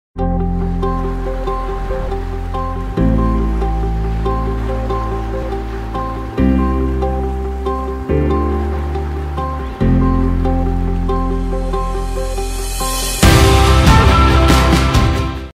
Awaken (Probuzení) je vůně roku 2020 od Yankee Candle
Krystalicky svěží a povznášející vůně čistoty ve které ucítíte slané mořské tóny, naplavené dřevo a lehký závan květin. Povzbudí a inspiruje vás k prožití a ocenění prosté krásy každého okamžiku života. Vůně Awaken (Probuzení) z limitovaná edice Scent of the Year 2020 vznikla ve spolupráci Yankee Candle s trendovými agenturami, sociology a umělci. Ti všichni inspirovali novou a unikátní vůni, která vystihuje aktuální společenské trendy a náladu letošního roku.